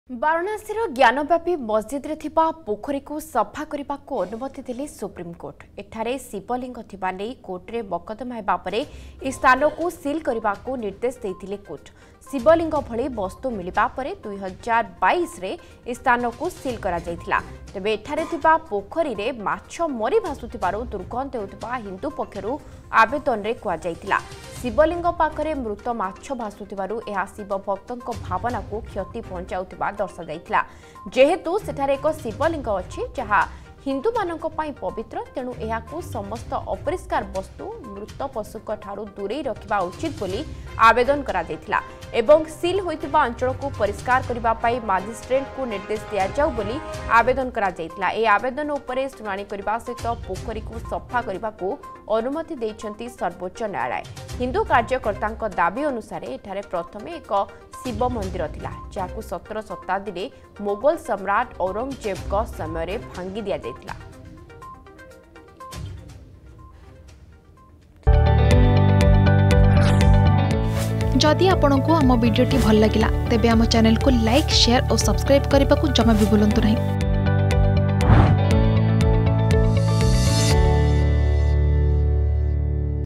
ଜ୍ଞାନବାପି ମସଜିଦରେ ଥିବା ପୋଖରୀକୁ ସଫା କରିବାକୁ ଅନୁମତି ଦେଲେ ସୁପ୍ରିମକୋର୍ଟ || Gyanvapi Mosque Case
ବାରାଣାସୀର ଜ୍ଞାନବାପି ମସଜିଦରେ ଥିବା ପୋଖରୀକୁ ସଫା କରିବାକୁ ଅନୁମତି ଦେଲେ ସୁପ୍ରିମକୋର୍ଟ ।